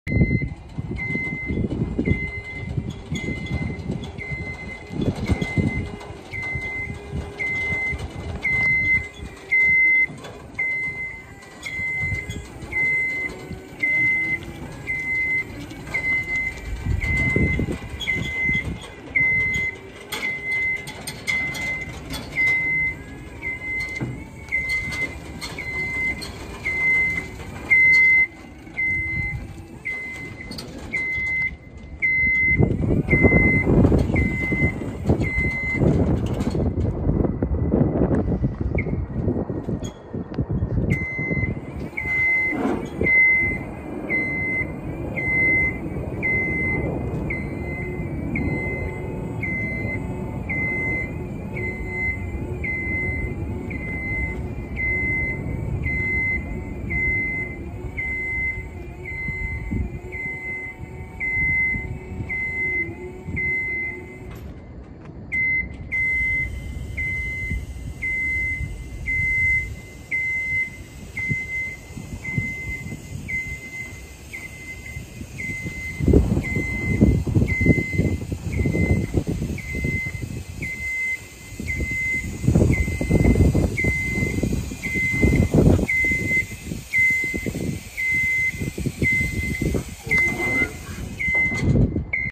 2016 GENIE GS2632 For Sale
Make: Genie
Model: GS-2632
Year: 2016
SN: GS3216P-144874
Electric
26 FT Height
Extendable Platform